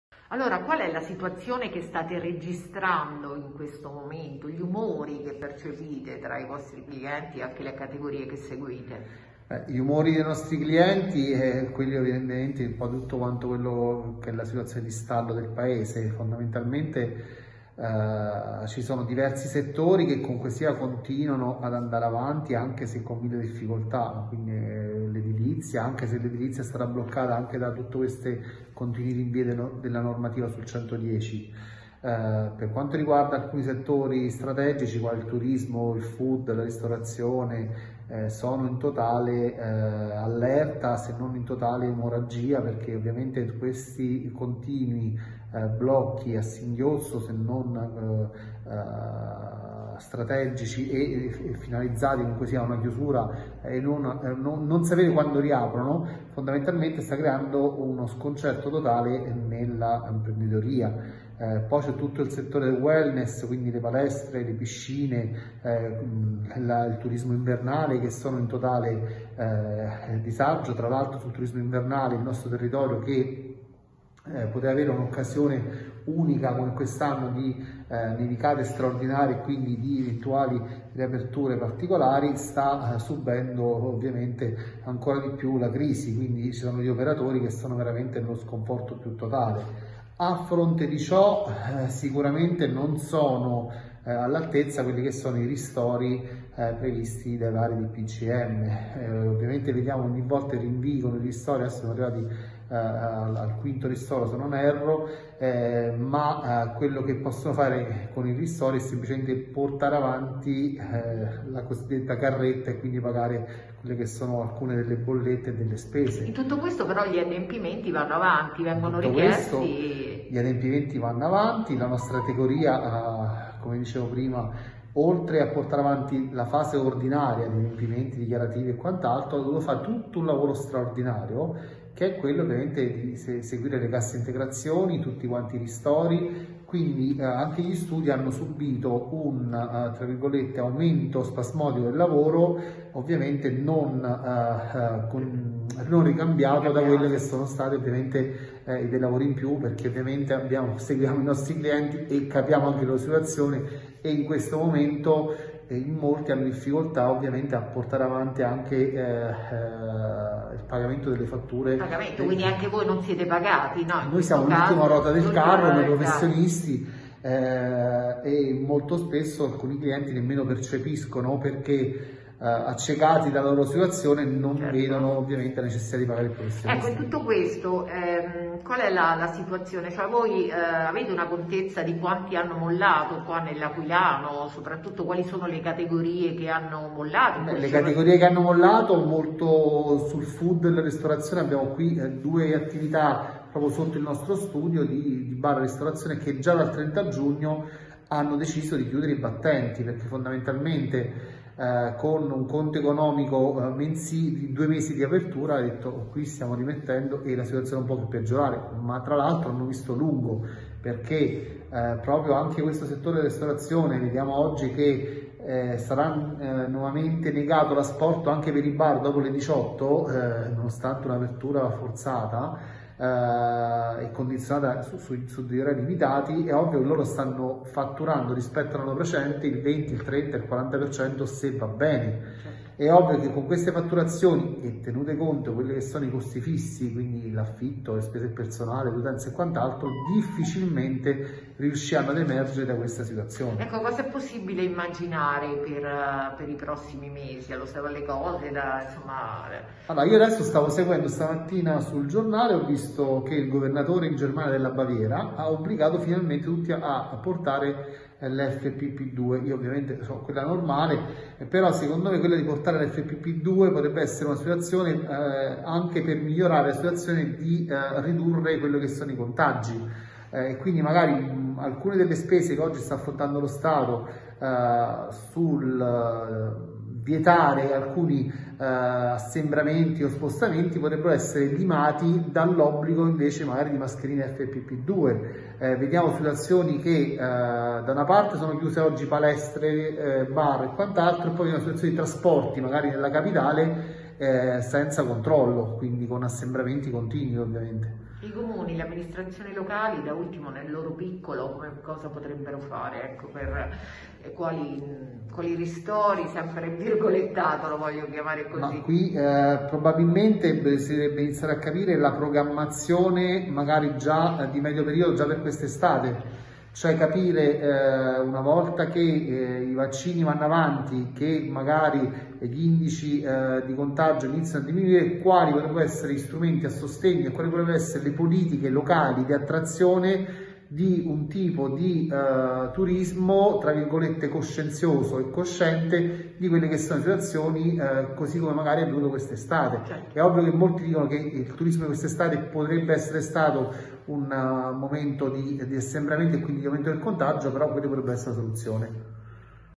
Commercialisti, Perrotti: "lo sconforto, food e ristorazione chiudono"
"Gli operatori sono nello sconforto più totale - dice a Laquilablog, Ettore Perrotti,  presidente dell'Ordine dei Commercialisti dell'Aquila -; con i ristori che arrivano si può solo portare avanti la carretta per pagare le spese".